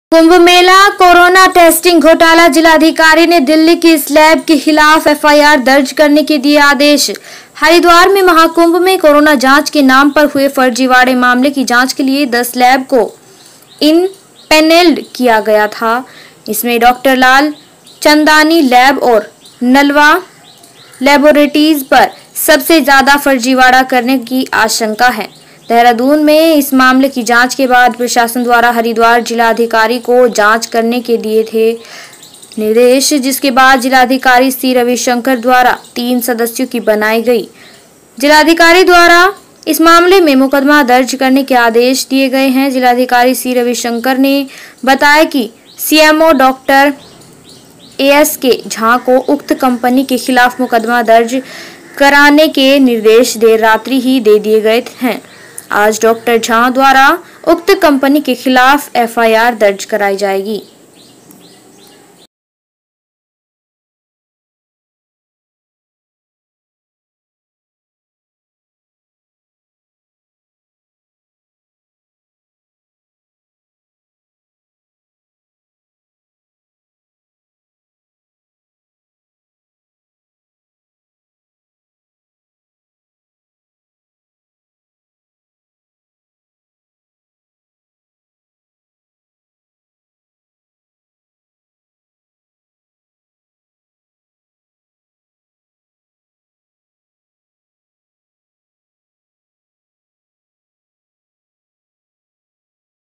कुंभमेला कोरोनाटेस्टिंग घोटाला जिलाधिकारी ने दिल्ली की इस लैब के खिलाफ एफआईआर दर्ज करने के दिये आदेश
लोकेशन-हरिद्वार

रिपोर्ट-अनिकेत

-कुंभ मेला कोरोना टेस्टिंग घोटाला। जिलाधिकारी ने दिल्ली की इस लैब के खिलाफ एफआईआर दर्ज करने के दिये आदेश

एंकर-हरिद्वार: कुंभ कोरोना जांच फर्जीवाड़े मामले में हरिद्वार जिलाधिकारी सी रविशंकर ने मुकदमा दर्ज करने के आदेश दिये हैं. इस मामले में दिल्ली की मैक्स कॉरपोरेट सर्विस लैब के खिलाफ मुकदमा दर्ज किया जाएगा. पंजाब के रहने वाले व्यक्ति द्वारा की गई शिकायत के बाद ये कार्रवाई की गई है.

हरिद्वार महाकुंभ में कोरोना जांच के नाम पर हुए फर्जीवाड़े मामले की जांच के लिए 10 लैब को इनपैनल्ड किया गया था. इनमें डॉ. लाल चंदानी लैब और नलवा लैबोरेट्रीज पर सबसे ज्यादा फर्जीवाड़ा करने की आशंका है. देहरादून में इस मामले की जांच के बाद प्रशासन द्वारा हरिद्वार जिला अधिकारी को जांच करने के दिए थे. जिसके बाद जिलाधिकारी सी. रविशंकर द्वारा तीन सदस्यों की बनाई. जिलाधिकारी द्वारा इस मामले में मुकदमा दर्ज करने के आदेश दिए गए हैं.। जिलाधिकारी सी रविशंकर ने बताया कि सीएमओ डॉ एस के झा को उक्त कंपनी के खिलाफ मुकदमा दर्ज कराने के निर्देश देर रात ही दे दिए गए हैं। आज डॉक्टर झा द्वारा उक्त कंपनी के खिलाफ एफ आई आर दर्ज कराई जाएगी।

ऐसे खुला कोरोना जांच घोटाला

हरिद्वार कुंभ में हुए टेस्ट के घपले का खुलासा पंजाब के रहने वाले एक LIC एजेंट के माध्यम से हुआ है. पंजाब के फरीदकोट के रहने वाले एक शख्स विपिन मित्तल ने हरिद्वार कुंभ में कोविड जांच घोटाले की पोल खोली. विपिन मित्तल के मुताबिक उन्हें उत्तराखंड की एक लैब से फोन आया था जिसमें उन्हें बताया गया कि ‘आप की रिपोर्ट निगेटिव आई है’. जिसे सुनते ही वे भौचक्के रह गए.
उन्होंने इस घटना की शिकायत आईसीएमआर से की और कहा कि उसके आधार और मोबाइल नंबर का फेक टेस्ट के लिए गलत इस्तेमाल हो रहा है। बाद में आईसीएमआर ने इस मामले का संज्ञान लेते हुए उत्तराखंड के स्वास्थ्य विभाग के एक वरिष्ठ अफसर को इसकी जानकारी दी। बताया गया है कि उत्तराखंड के इस अफसर ने कुंभ मेले में टेस्टिंग की जिम्मेदारी निभाने वाली प्राइवेट लैब की रिपोर्ट्स की जांच की। जब शुरुआती जांच में ही लैब की ओर से कई और लोगों को फेक रिपोर्ट्स देने की बात सामने आई, तब उन्होंने मामले में विस्तृत जांच के आदेश दिए